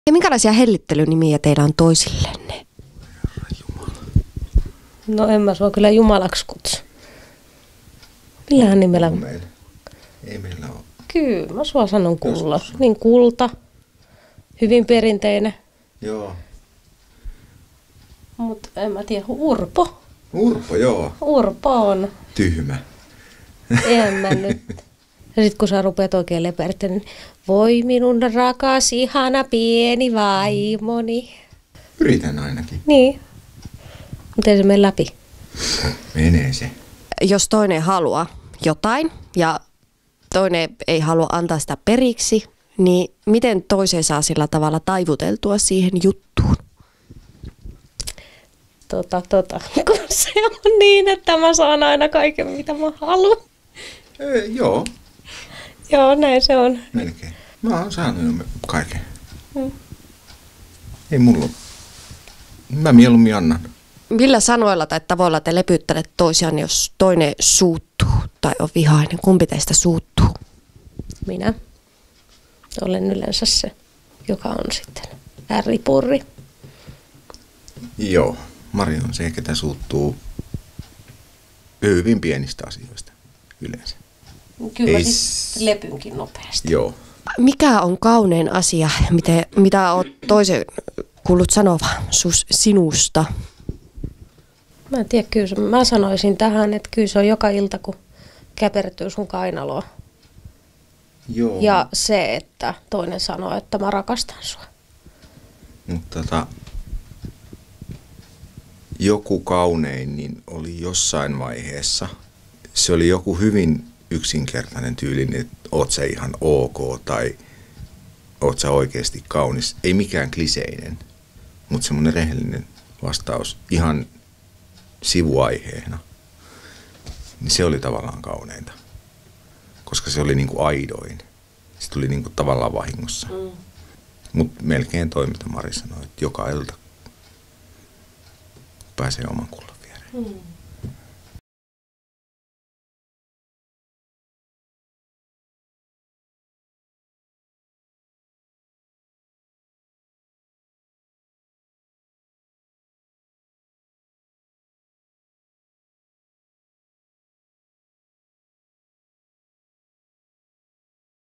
Lohjalaiset Asko ja Mari ovat kuin Jing ja jang: "Olen saanut jo kaiken, joten mieluummin annan!"
Lohjalaiset Mari, 32, ja Asko, 42, Tenhula tulivat kansalle tutuksi viime kuussa nähdyssä Unelmahäät-jaksossa.
Umpirakastanut pariskunta paljastaa Seiskalle suhteensa salat.